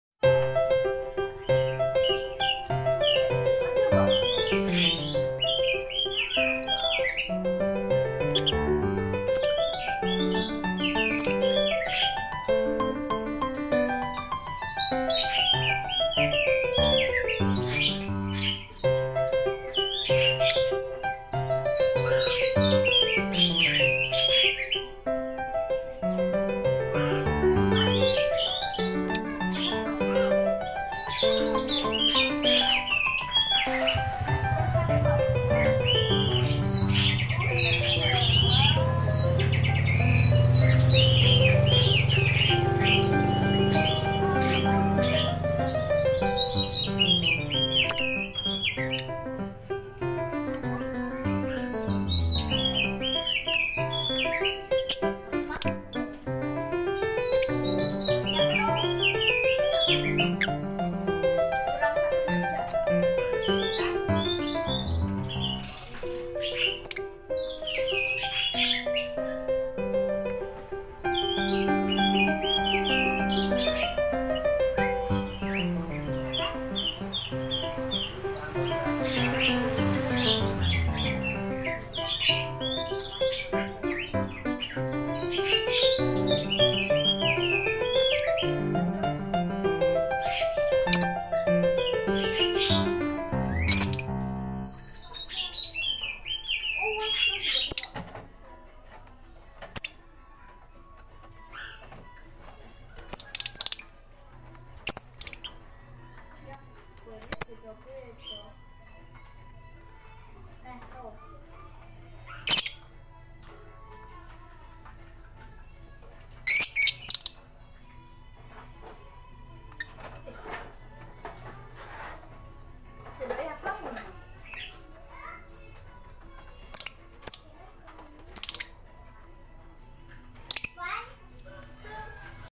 Lovebird di kandang minimalis!kicau mania
Burung#peliharaan#lucu# minimalis di teras samping rumah